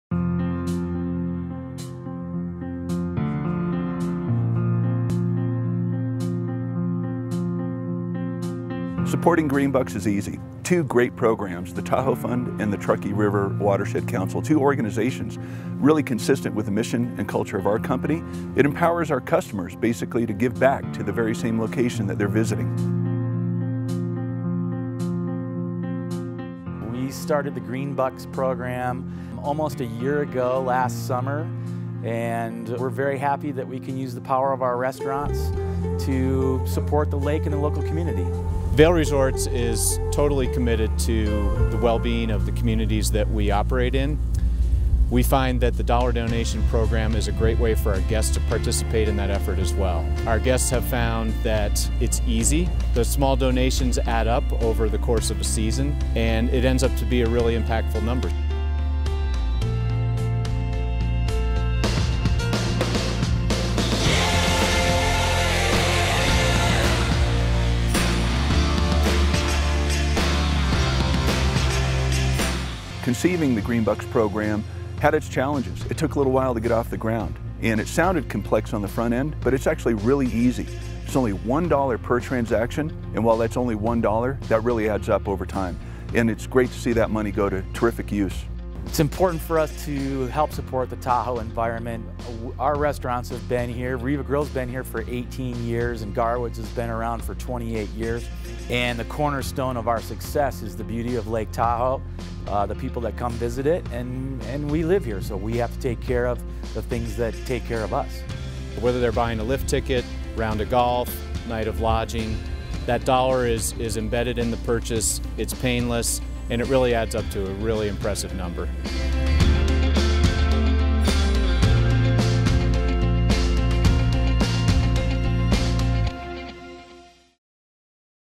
$1 for Tahoe, $ 1 for Truckee
Businesses across the Tahoe Region are adding $1 donations to lift tickets, hotel rooms, restaurant tabs and other items to raise funds for environmental projects of the Tahoe Fund and Truckee River Watershed Council. We invite you to join our efforts!